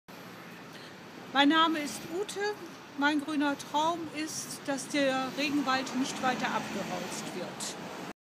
Ute from Germany
Ute's Green Dream that the rainforests can be sustained and destruction will be stopped.